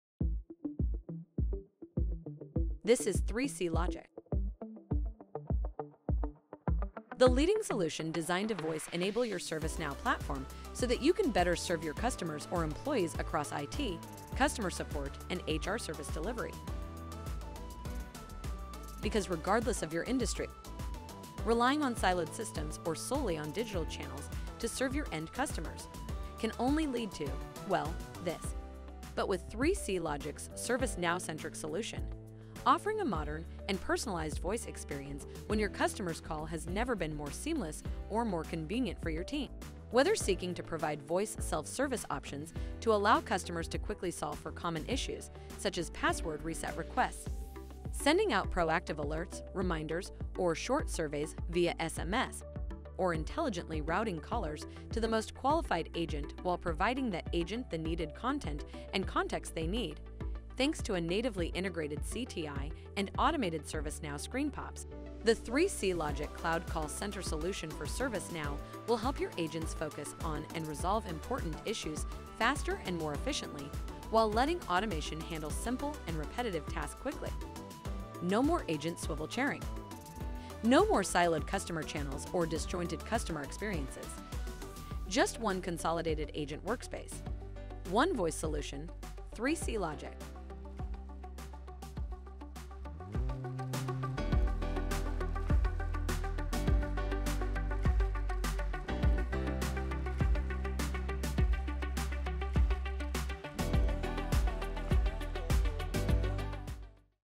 3CLogic Cloud Contact Center Solution for ServiceNow ITSM, CSM, and HRSD
Enhance ServiceNow ITSM, CSM, or HRSD with 3CLogic’s leading CTI integration and voice-enabling solution to empower agents to resolve critical issues more efficiently while allowing automation to swiftly handle repetitive tasks.

Deliver seamless, personalized experiences with voice self-service to allow customers to address common issues, integrated SMS to send proactive alerts or short surveys, and intelligent call routing to streamline customer and employee workflows — all from the ServiceNow agent workspace.